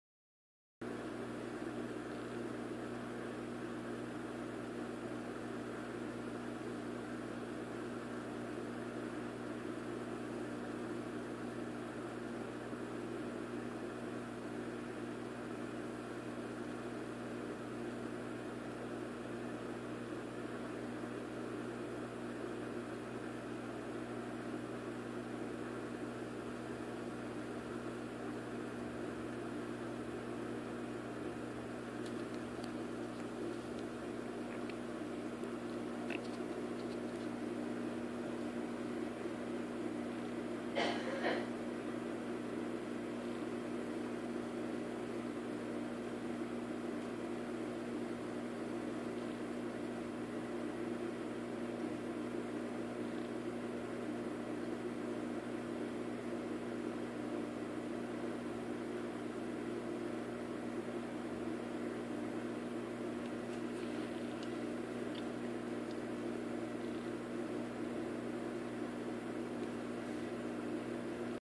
emerald crab eating
just a short video of my new emerald crab eating of my green button polyp rock. this thing is awesome!